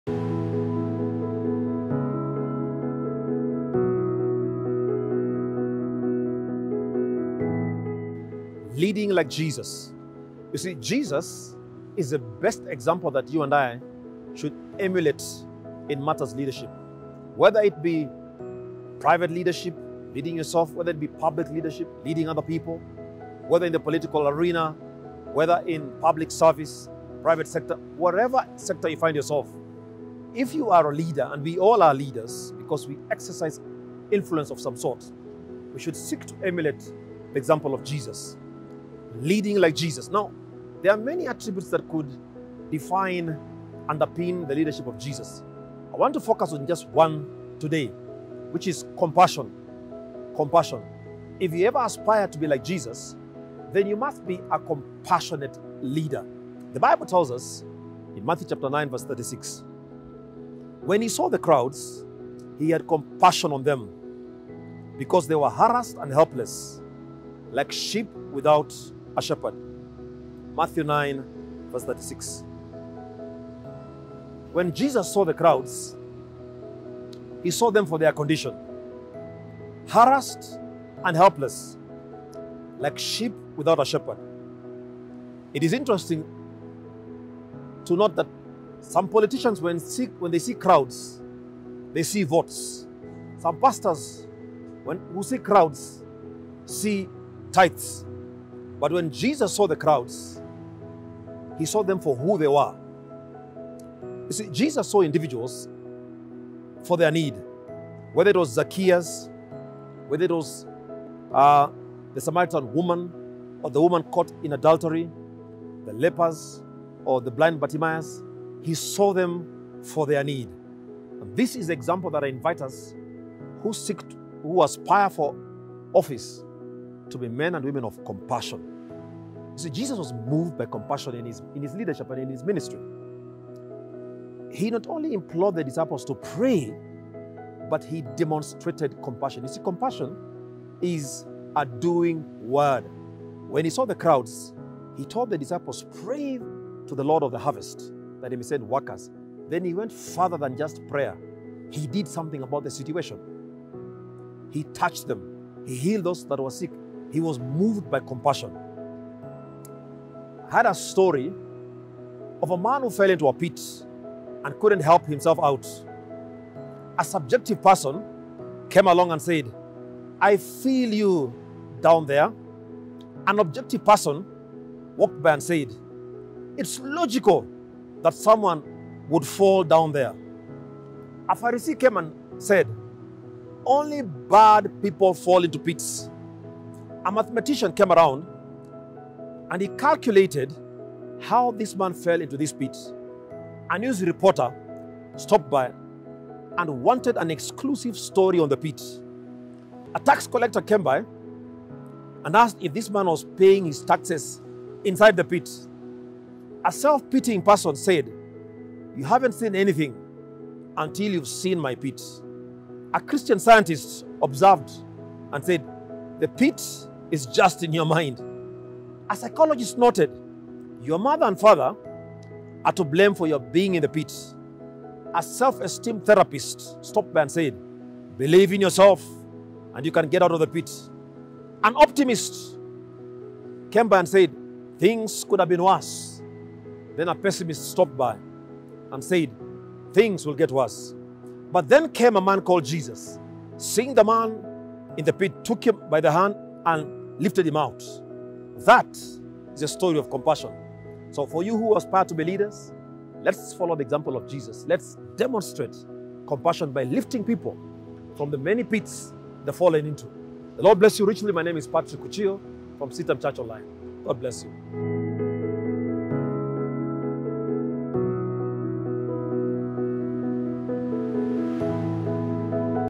Leading Like Jesus; Compassion - Rev. Patrick Kuchio | CITAM Church Online
In pursuit of being good leaders, we must seek to emulate the example of Jesus and at the heart of his leadership, is compassion for people.

Today on WisdomWednesday Rev. Patrick Kuchio takes us through 'Leading Like Jesus; Compassion'.